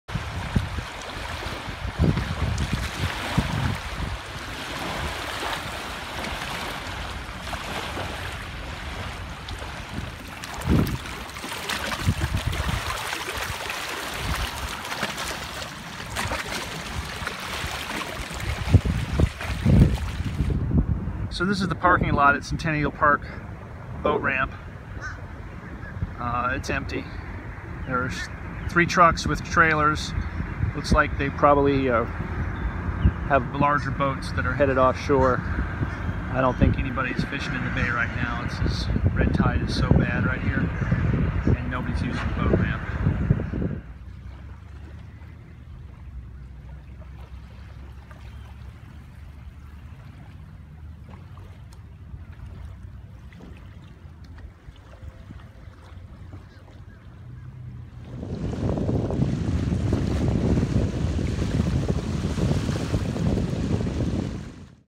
Red Tide Update, Sarasota Bay, Aug 21, 2018
Dead fish continue to wash up along Sarasota Bay.  We checked out Centennial Park and Bayfront Park today for this update.